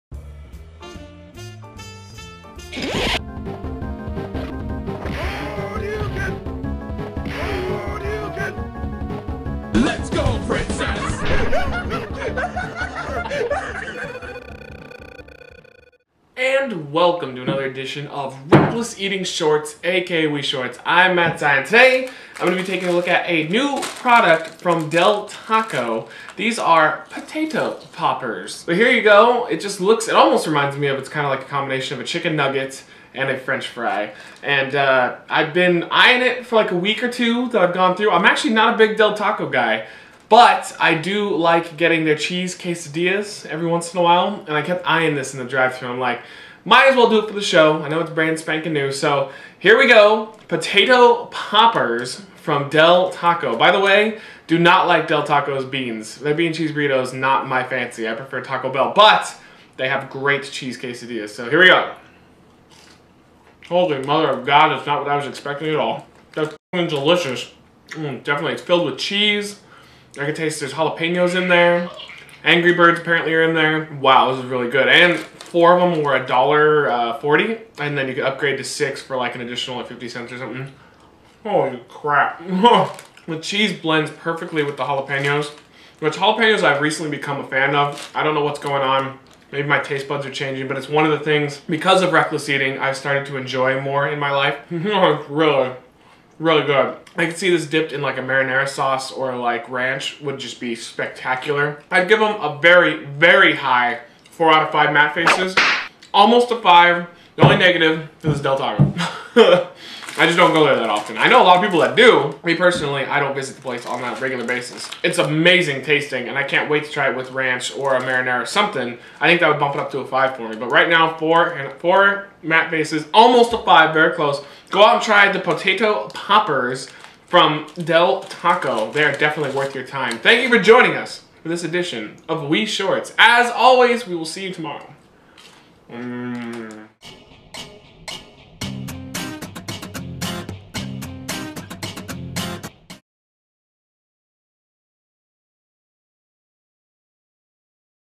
WE Shorts - Del Taco Potato Poppers
Today on we shorts Matt Zion reviews potato poppers from Del Taco!